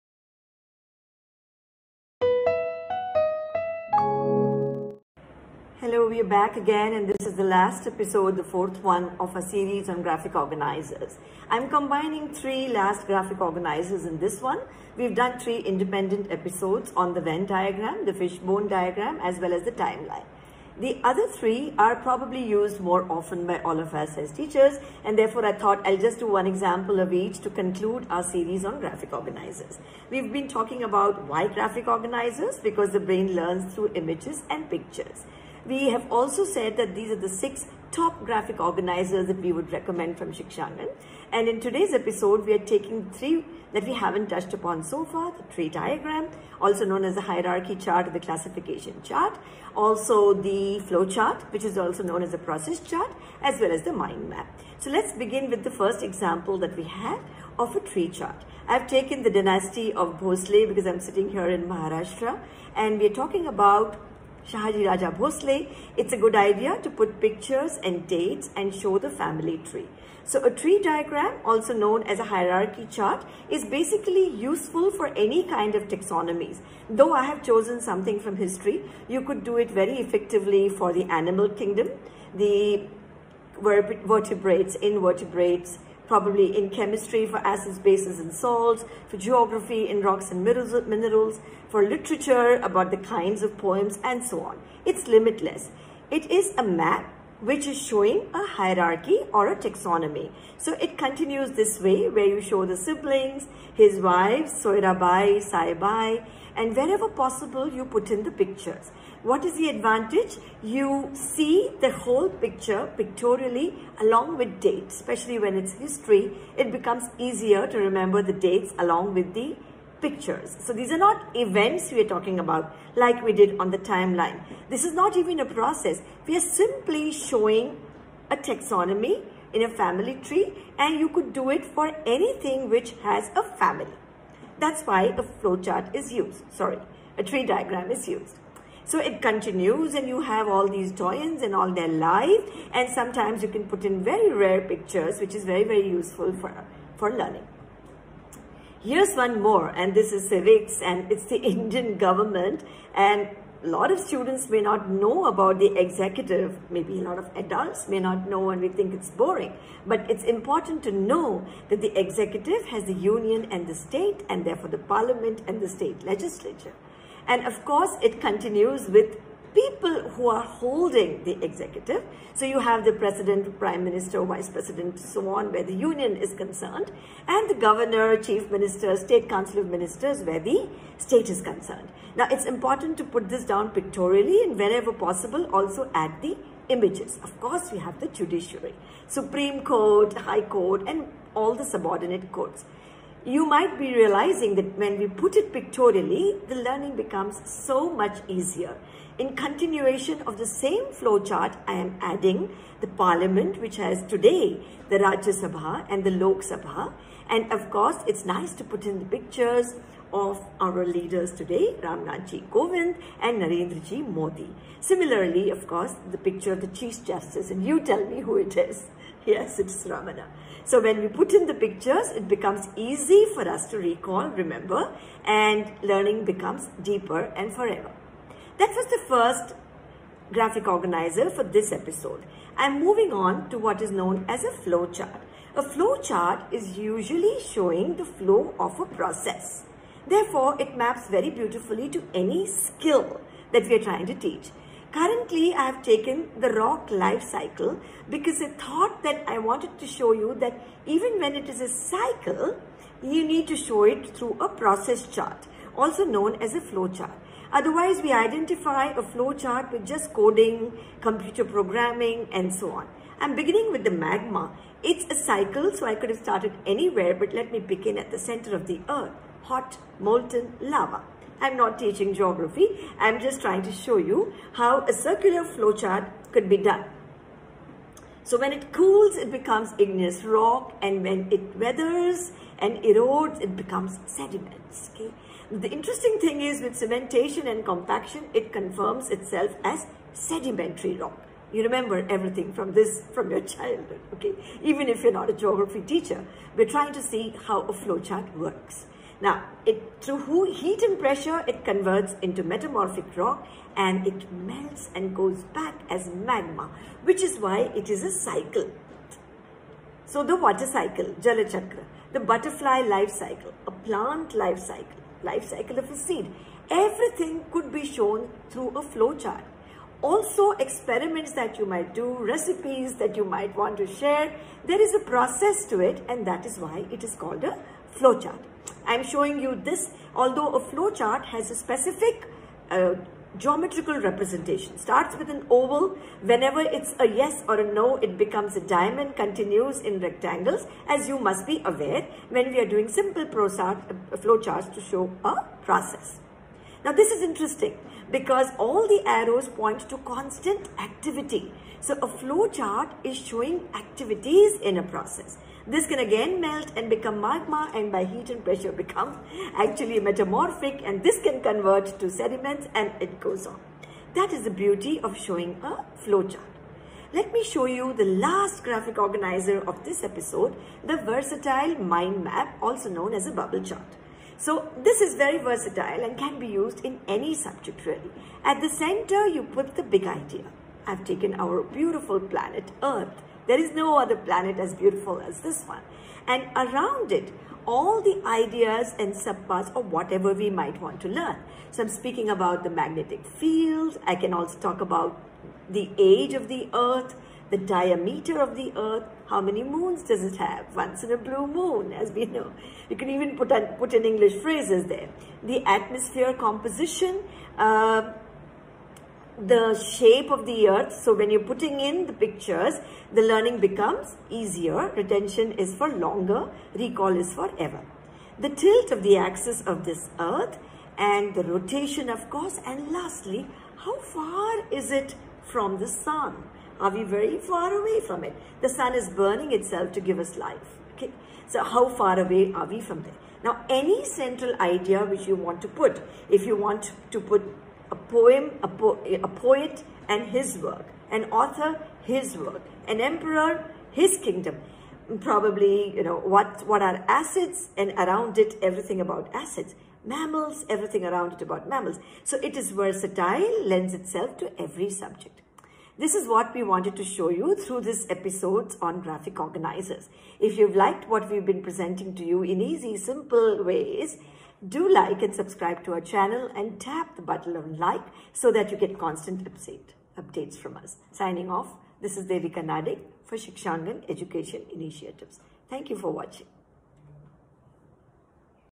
Flow Chart / Tree Diagram / Mind Map | Episode - 4 | Presented by Devika Nadig
These are familiar Graphic Organisers for most educators. However , Shikshangan presents the old wine in a new bottle - by giving relevant examples from the syllabus .Mastering these three graphic organisers will help you to create the right perspective for understanding most concepts